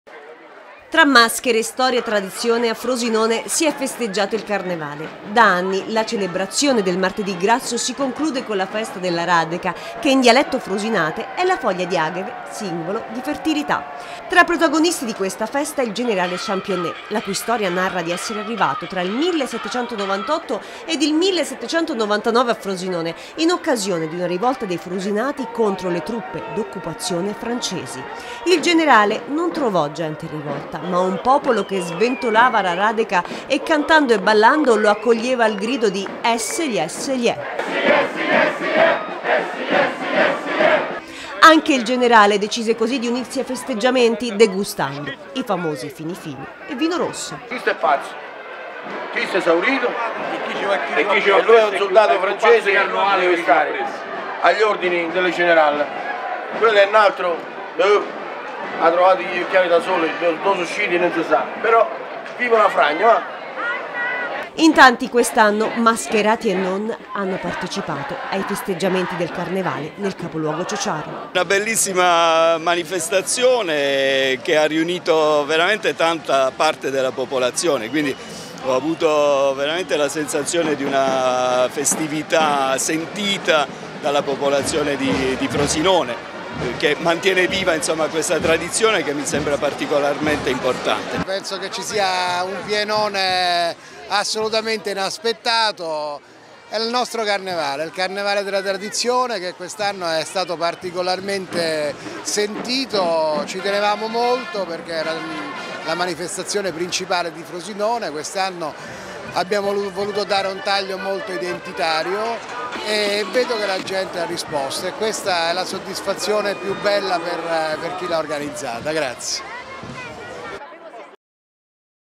FROSINONE: La Radeca del 21-02-2023
Un successo di pubblico mascherato e non a Frosinone in occasione dello storico Carnevale con annessa festa della Radeca.